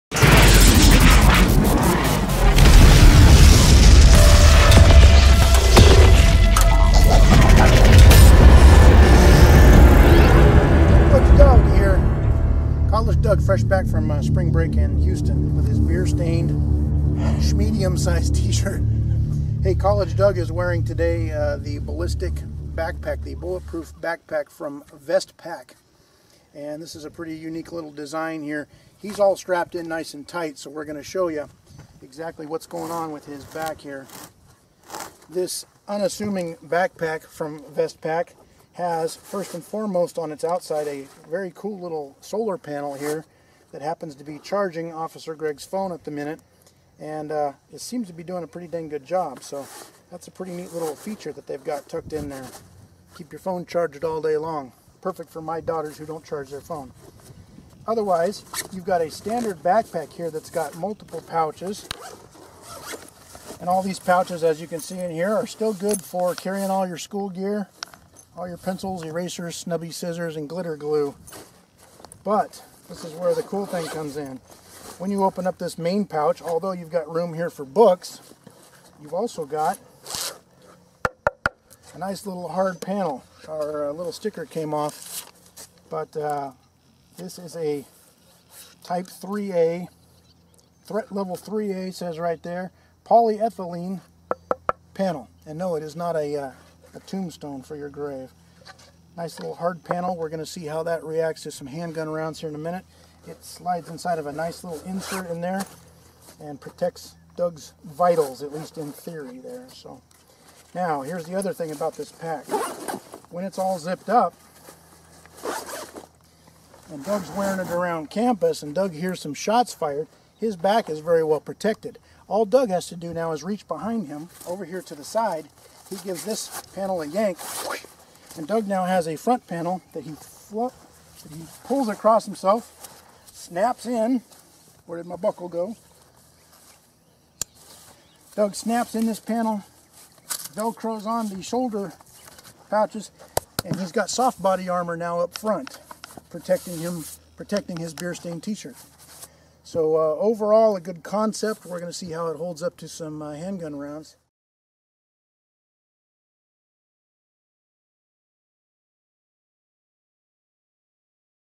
The VESTPAK™ Bulletproof Backpack- Concept.
The VESTPAK™ is a bulletproof backpack for Civilians, law enforcement, and smart people in general. Here’s the thing though, aside from having a NIJ Level IIIA panel in the back of the backpack, there’s the patented EASY PULL™ tab on the side of the pack that deploys a wrap-around frontal NIJ level IIIA panel giving you a full frontal and rear IIIA protection.

It is the coolest thing we have ever seen! In seconds, you are fully armored against most handgun and shotgun threats. All vital areas are protected and all of this protection is stored in an attractive and modern.

"We Created the VESTPAK Bulletproof Backpack with one simple yet vital purpose in mind: To protect lives".

Follow us @USAFEamerica